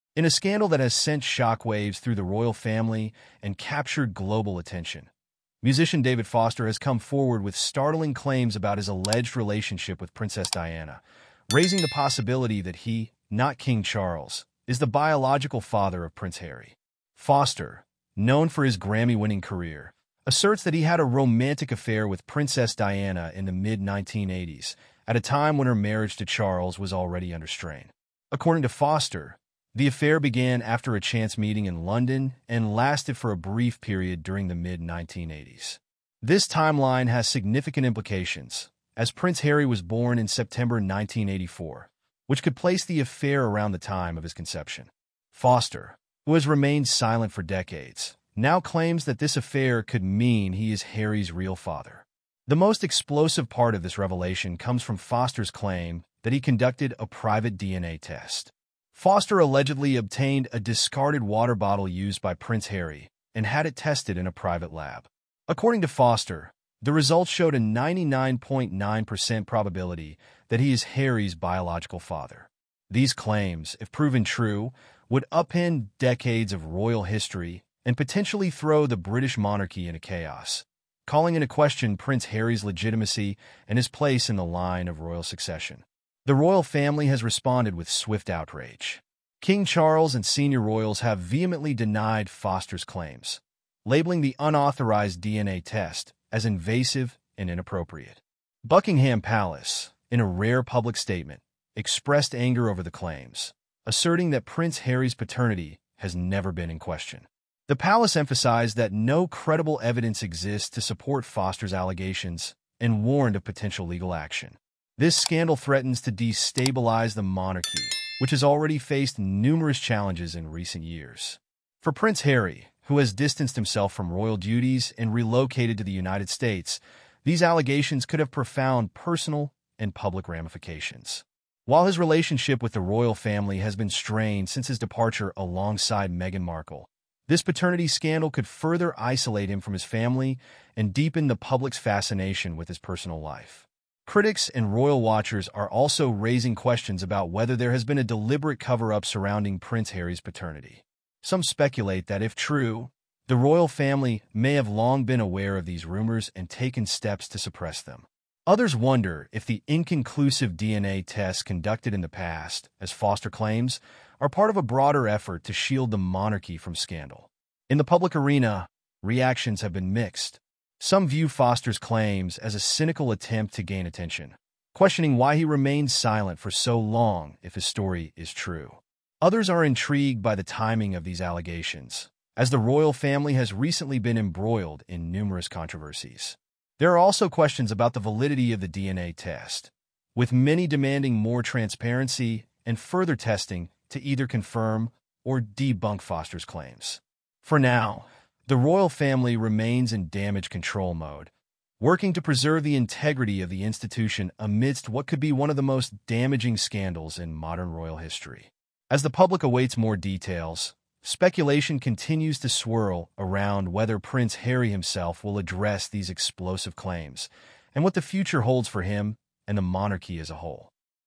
King Reveals Harry’s True Father: Strips Him of Title and Kicks Him Out!
In a scandal that has sent shockwaves through the royal family and captured global attention, musician David Foster has come forward with startling claims about his alleged relationship with Princess Diana, raising the possibility that he, not King Charles, is the biological father of Prince Harry. Foster, known for his Grammy-winning career, asserts that he had a romantic affair with Princess Diana in the mid-1980s, at a time when her marriage to Charles was already under strain.